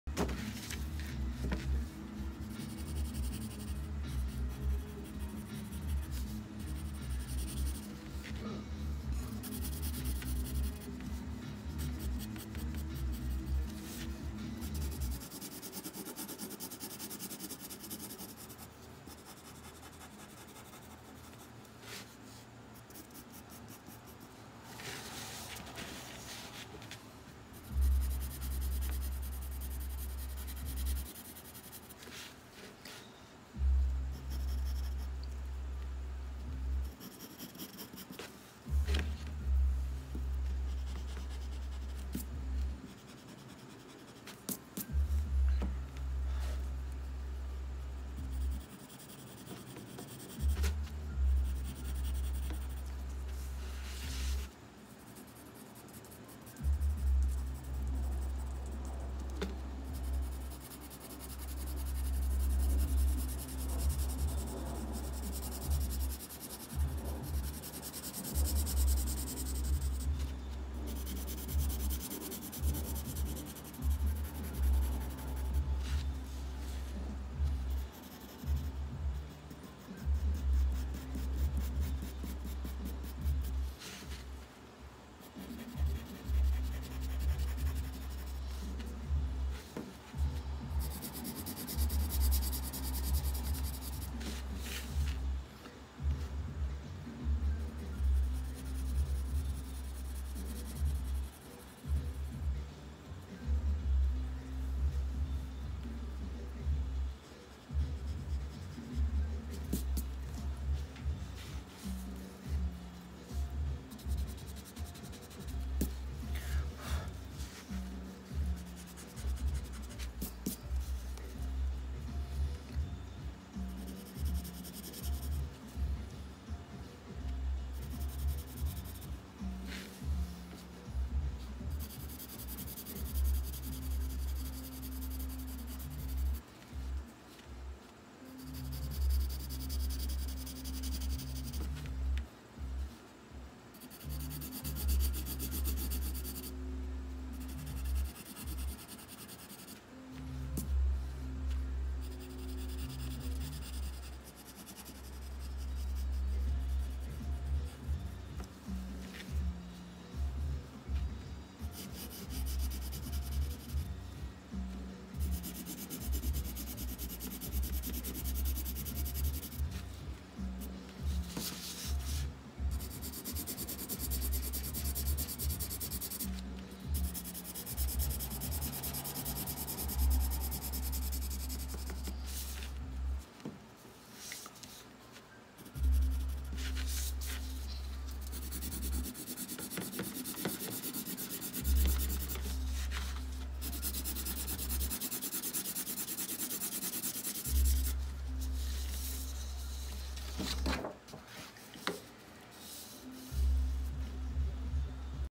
Frankenstein’s Monster original scratchboard art Part 3
This is part 3 of 3 short process videos I took while working on the frankenstein preliminary pencil sketch to the scratchboard for the upcoming Fantasy Art Art Expo.